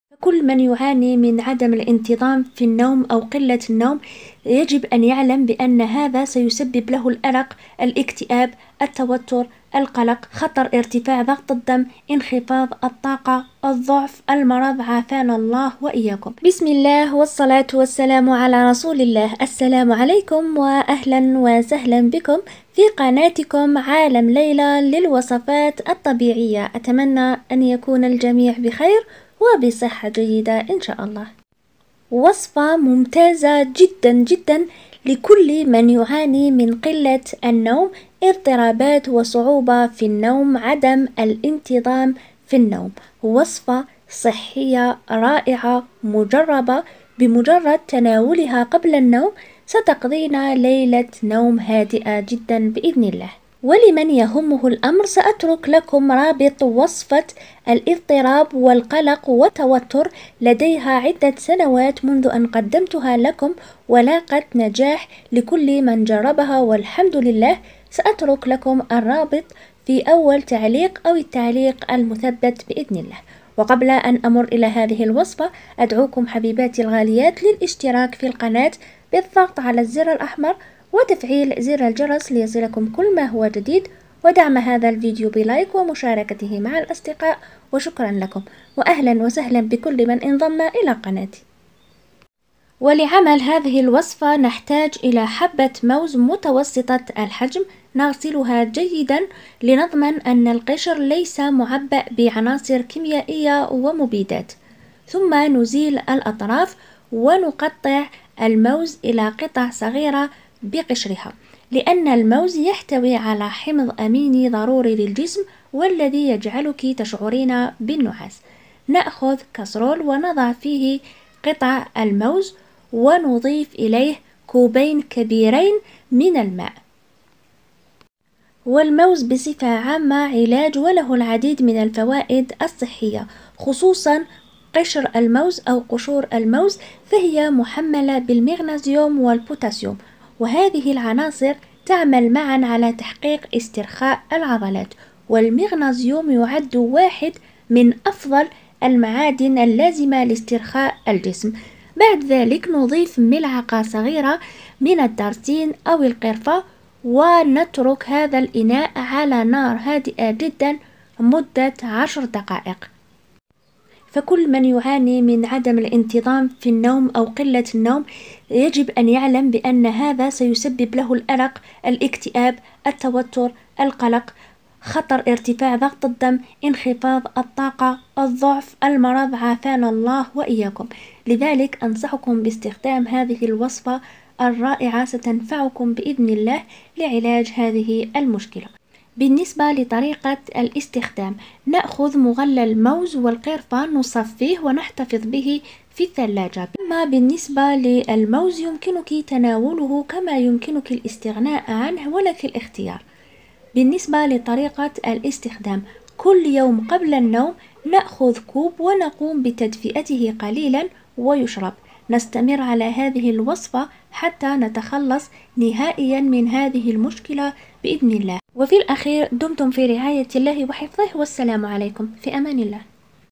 وصفة فعالة لعلاج الأرق وقلة النوم,كوب قبل النوم وتمتعي بليلة نوم هادئة وصحية,منوم ومهدئ طبيعي.
تحياتي للجميع🎀